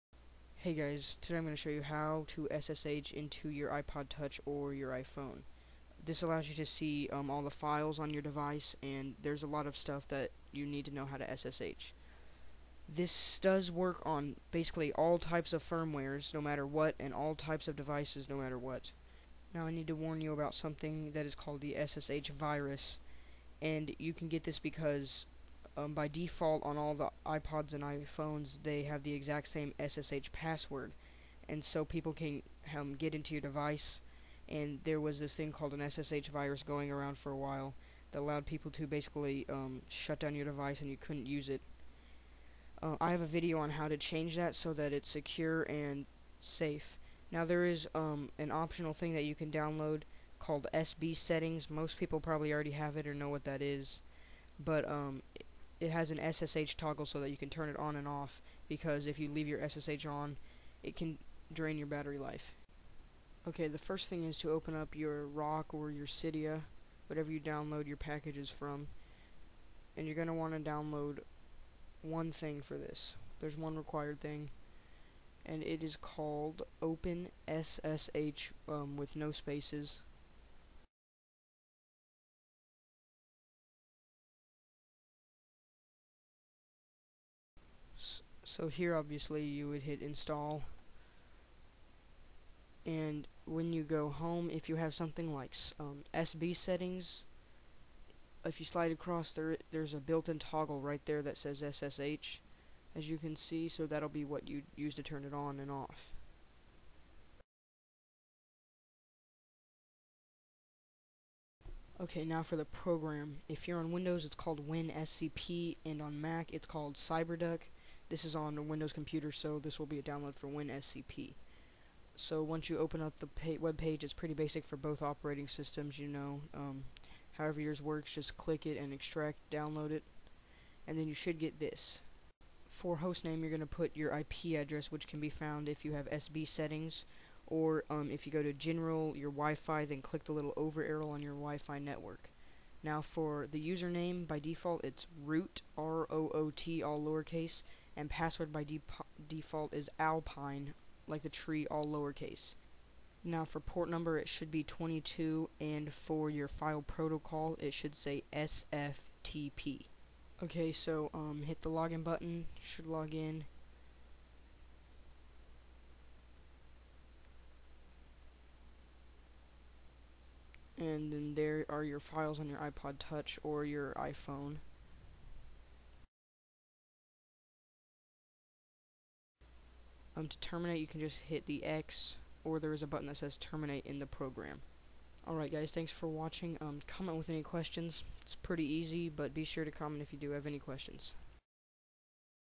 How To SSH on iPod Touch/iPhone Mac or Windows Any Firmware
Quick and easy tutorial on how to ssh into your device. Allows you to see your devices files and many things require SSHing.

Download link on the top right. The user interface is different then what you saw in the video. But they are close enough. Not hard to figure out.